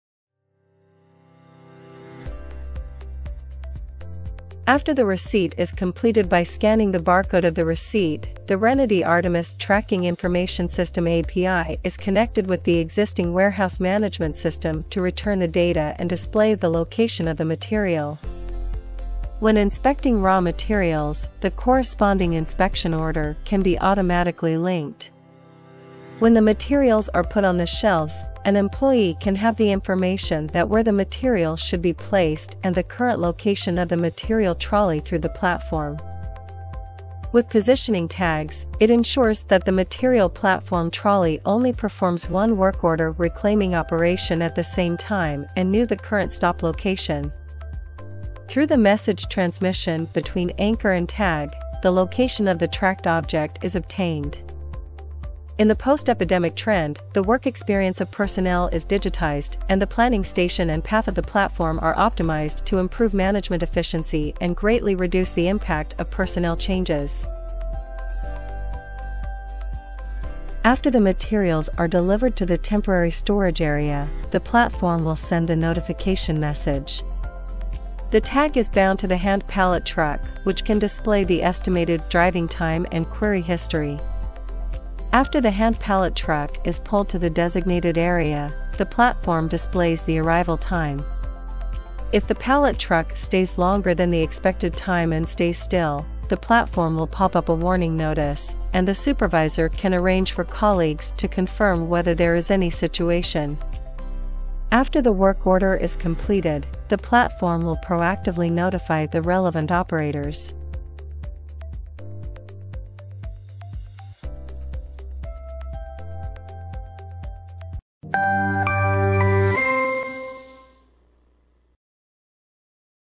Artemis-intelligent warehouse management system
As businesses around the world speed up the adoption of Industry 4.0, IoT has now become a major investment for businesses making the digital transformation. In IoT scenarios, RTLS (Real-time Location System) indoor positioning system is a highly valued technology in site management as it helps businesses to significantly increase management efficiency and cut manpower cost. Many technologies are capable of RTLS indoor positioning, including Bluetooth, RFID, UWB and ZigBee. However, UWB (Ultra-Wideband) used by RENITY ARTEMIS has the highest positioning precision (within 30cm) and stability. It also has high penetration depth and greatly resistant to interference. Armed with such advantages, ARTEMIS can provide customized IoT applications for different industries, further boosting customers’ production efficiency.